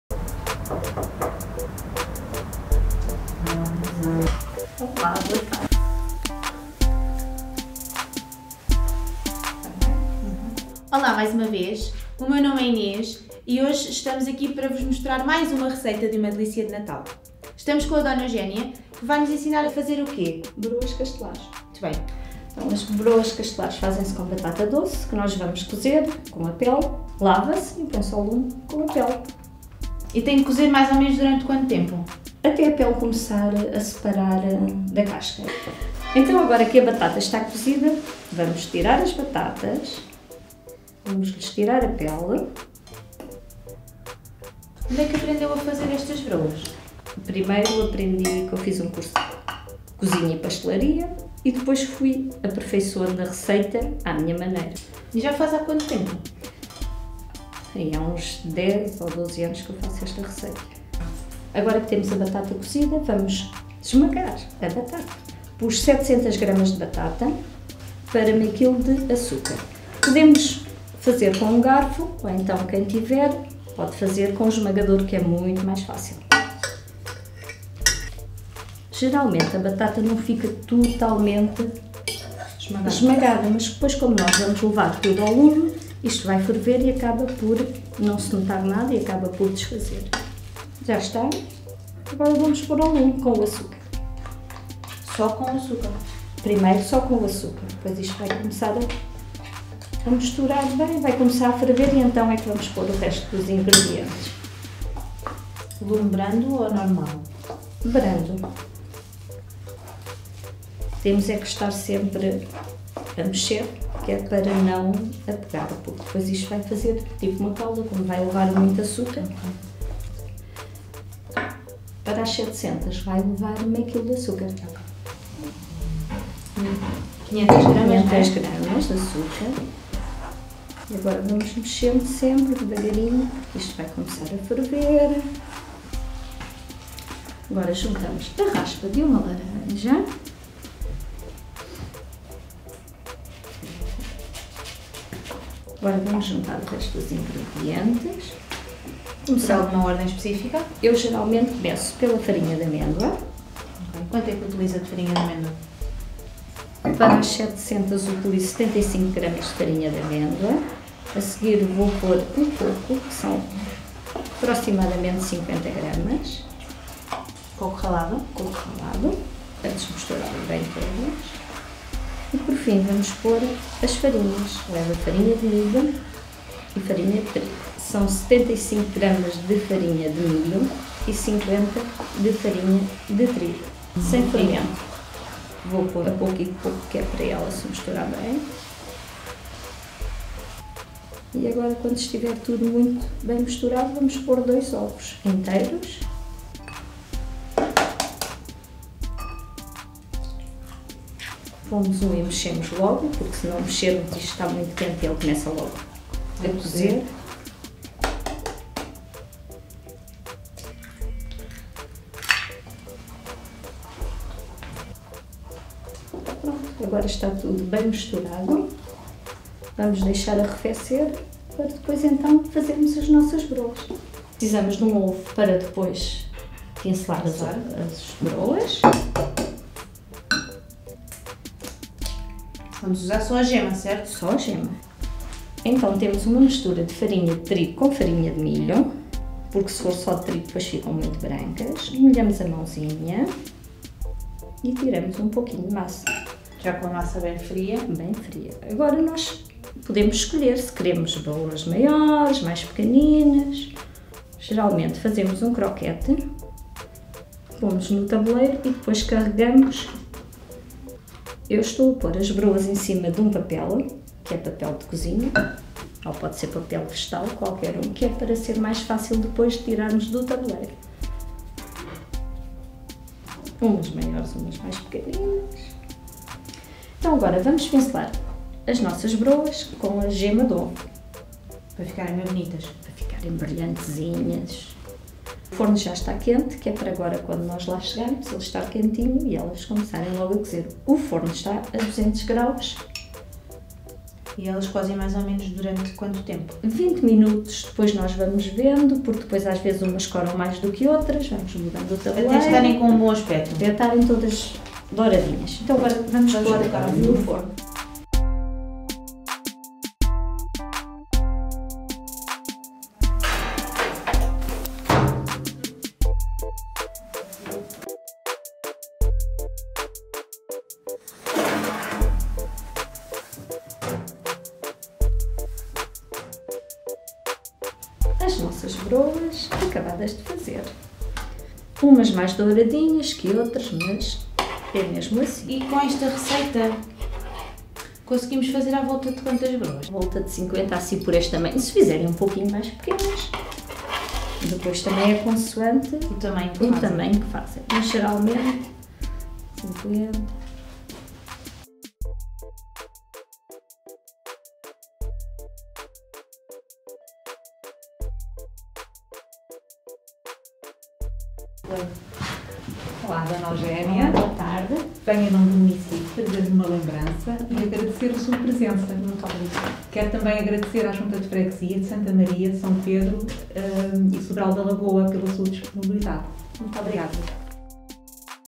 Delícias de Natal • Receita "Broas Castelares" (Santa Maria, São Pedro e Sobral da Lagoa)
Esta é a quarta receita de Natal que preparamos para si.
Sabe fazer Broas Castelares?  A D. Eugénia, da freguesia de Santa Maria, São Pedro e Sobral da Lagoa, ensina como fazer. Bom apetite!